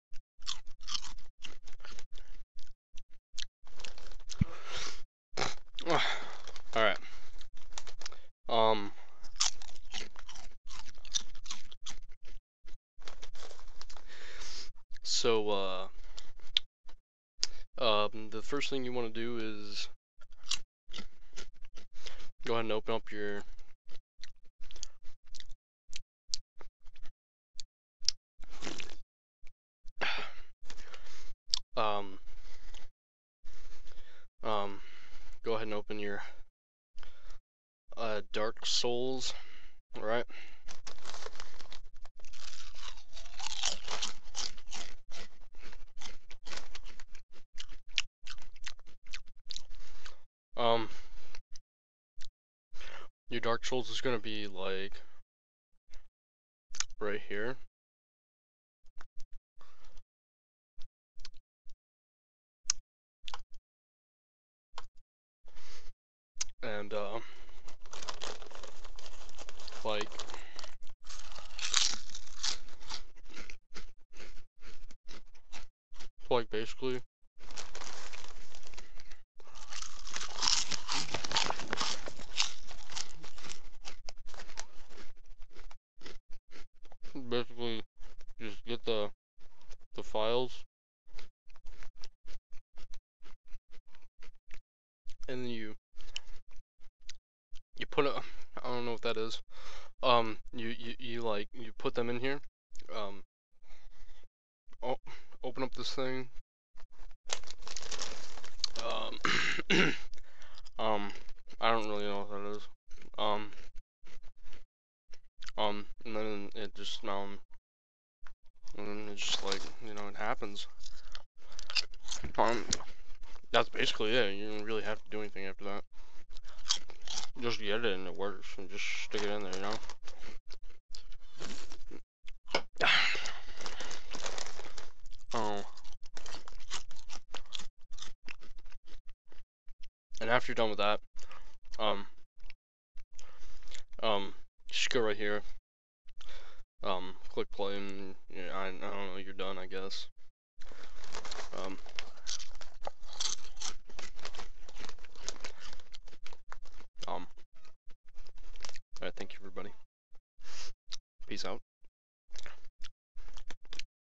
How to Install Dark Souls Fix and DSC PROPERLY
Step by step instructions on how to get your dark souls working properly with DS fix and DSC fix